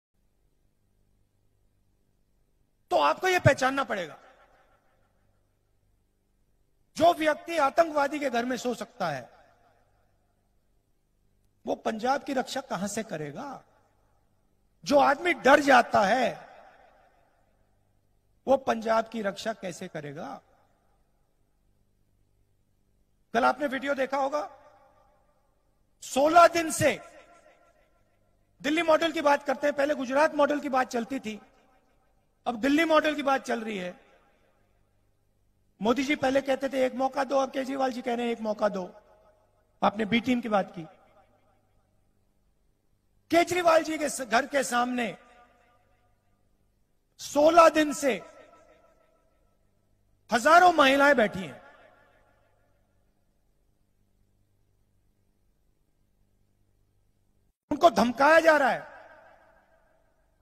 राहुल गांधी बोले 'जो व्यक्ति (केजरीवाल) आतंकवादी के घर सो सकता है, वो पंजाब की रक्षा कैसे करेगा'
कांग्रेस नेता राहुल गांधी बोले 'जो व्यक्ति (केजरीवाल) आतंकवादी के घर सो सकता है, वो पंजाब की रक्षा कैसे करेगा'
Rahul Gandhi